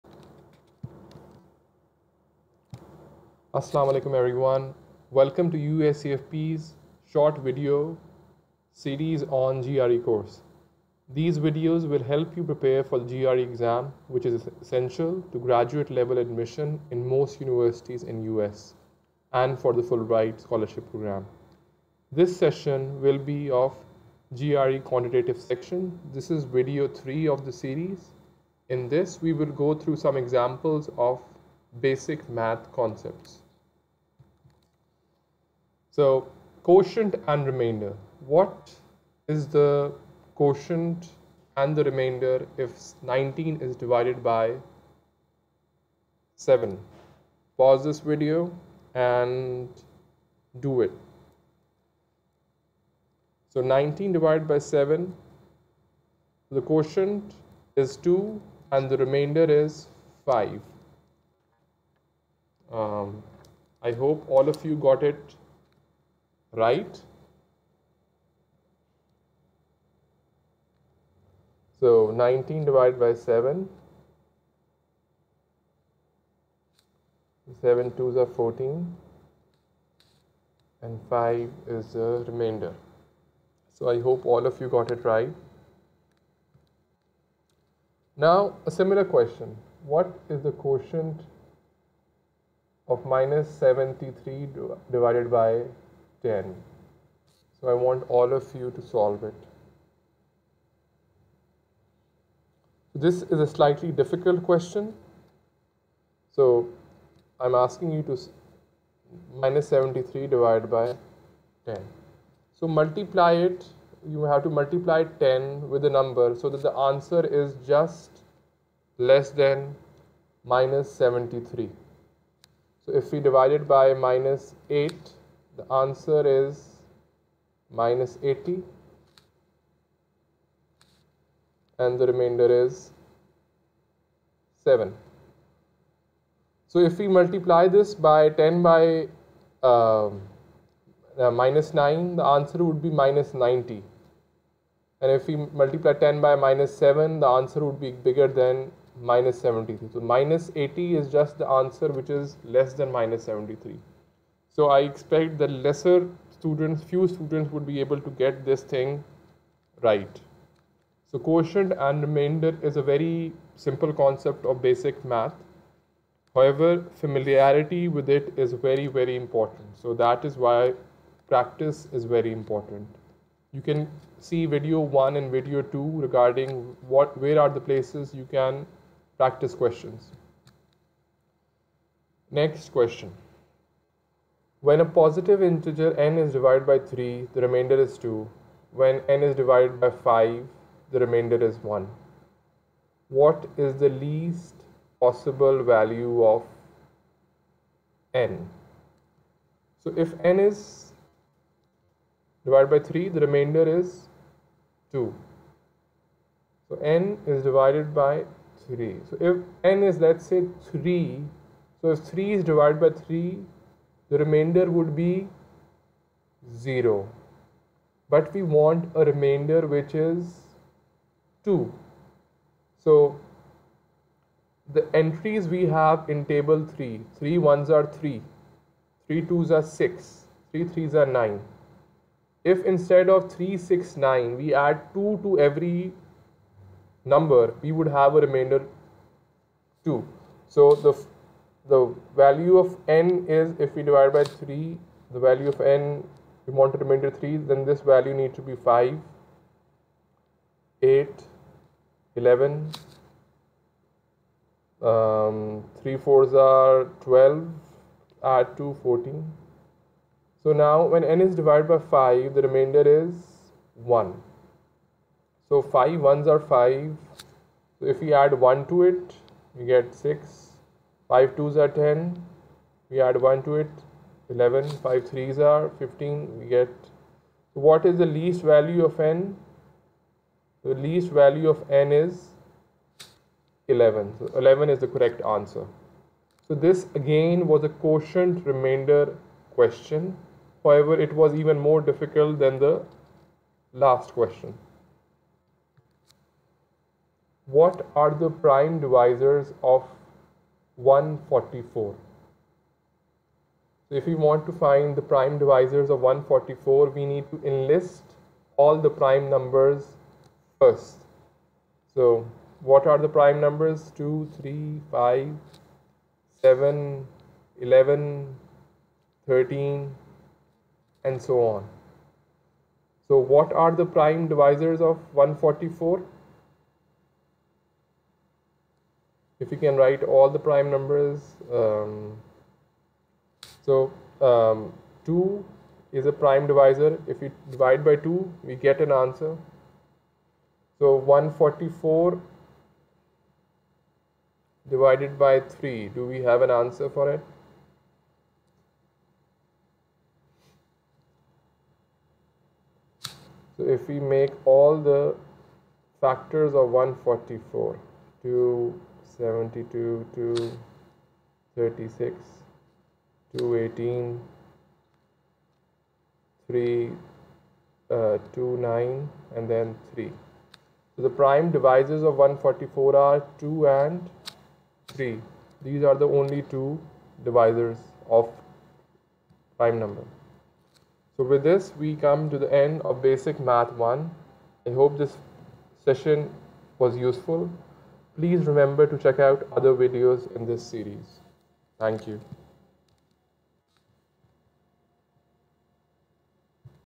GRE Quantitative | Part 3 | USEFP
Part 3 of the GRE Quantitative video discusses basic mathematics concepts as Dr. Ali Hussain Kazim shares tips to solve quotient and remainder questions.